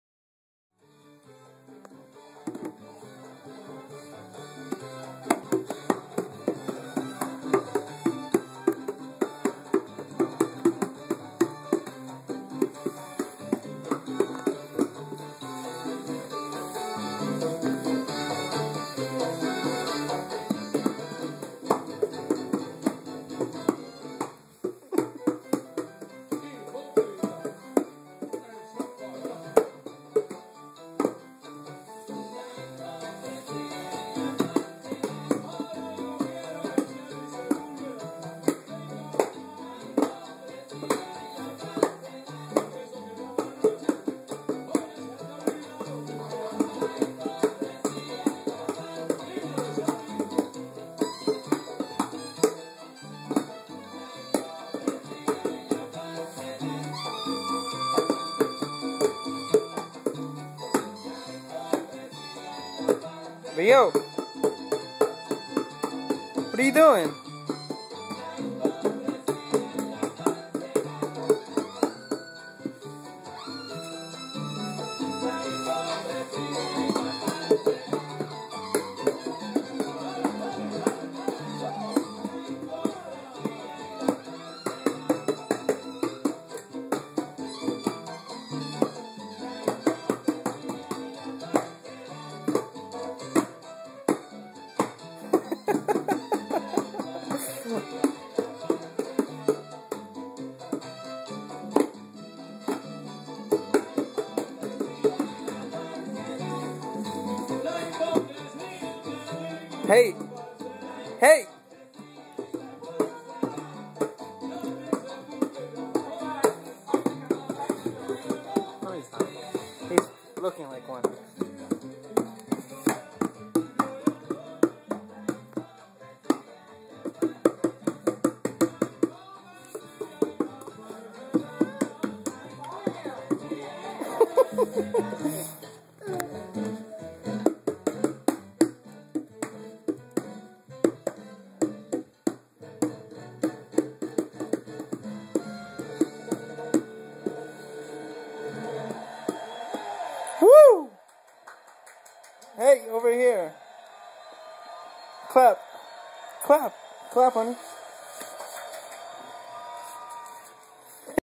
16 months and getting the bongos beat (finally)
Leo is getting the feel of the beat on his toy bongos.  And he's the only one allowed to use them.  Take a bow.  He's 16 months in this video.